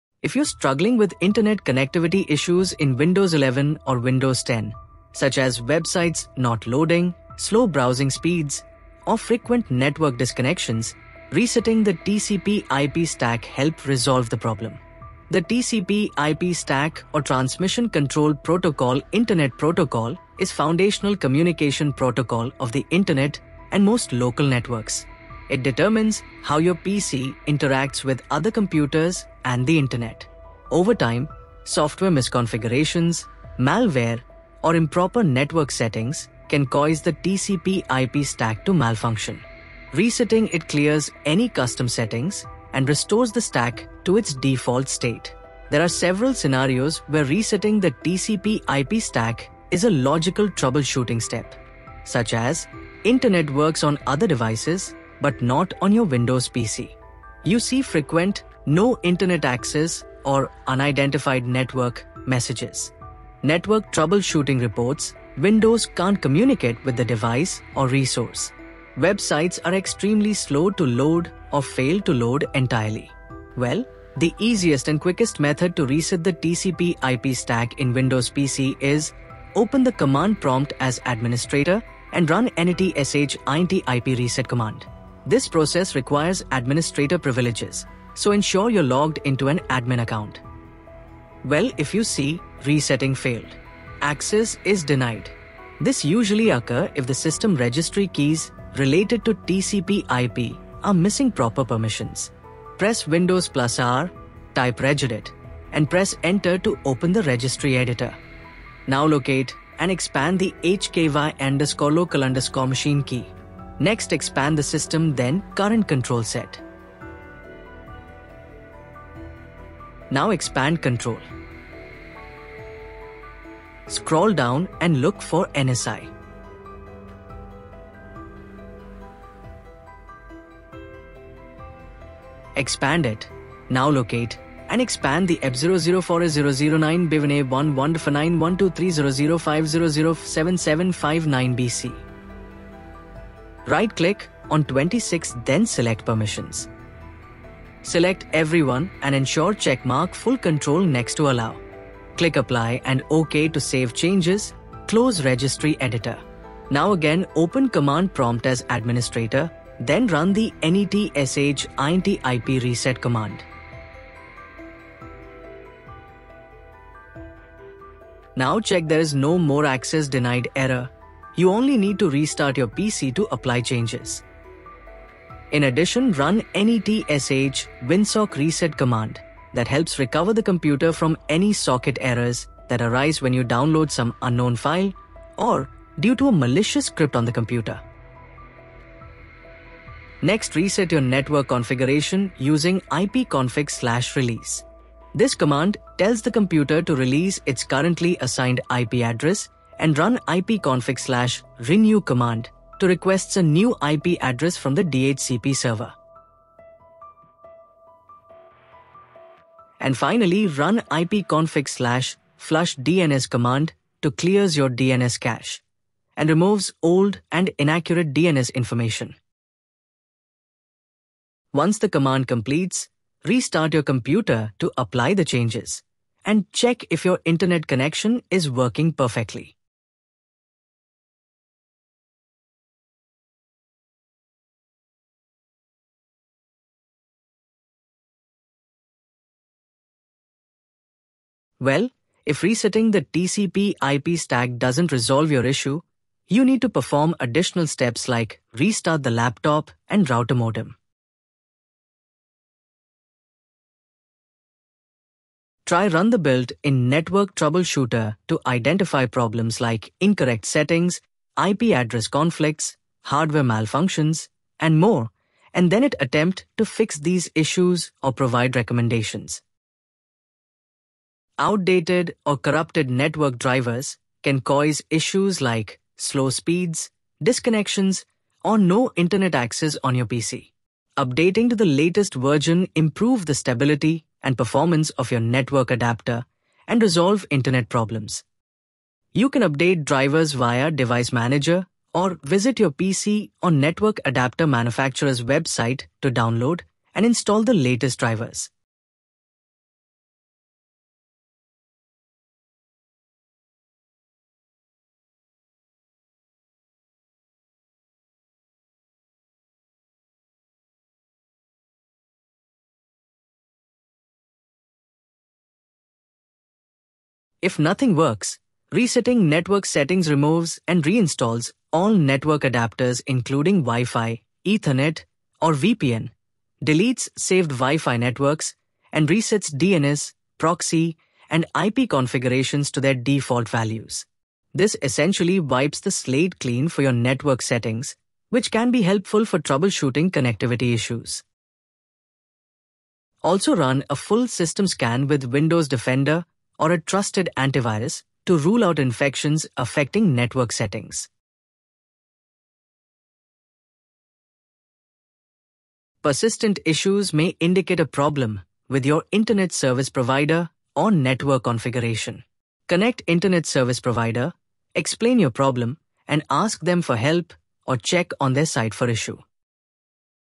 How to Reset TCP/IP Stack in Windows 11/10 | Fix Internet Connection Problems quickly
Over time, software misconfigurations, malware, or improper network settings can cause the TCP/IP stack to malfunction. Resetting it clears any custom settings and restores the stack to its default state. In this video, you'll learn how to reset the TCP/IP stack in Windows PC and fix internet connection problems quickly.

What is the TCP/IP stack?
The TCP/IP stack is the core protocol that allows your PC to communicate over networks and the Internet. When it gets corrupted or misconfigured, it can lead to no internet access, slow speeds, or DNS issues. Read detail in written article

When Should You Reset the TCP/IP Stack?
Internet works on other devices but not on your Windows PC.
You're getting IP conflict or DNS-related errors.
Websites load very slowly or not at all.
You’ve recently removed malware or adware affecting network settings.
Troubleshooter suggests "reset the network adapter" or similar fixes.

In this Video tutorial, I’ll show you:
✔️ What the TCP/IP stack is and why it matters
✔️ How to reset the TCP/IP stack using Command Prompt
✔️ How to run the Winsock reset for a complete network fix
✔️ What to do if you see "Access Denied" errors
✔️ Additional tips if the reset doesn’t work

💡 Whether you're on Windows 11 or Windows 10, this fix works for both versions!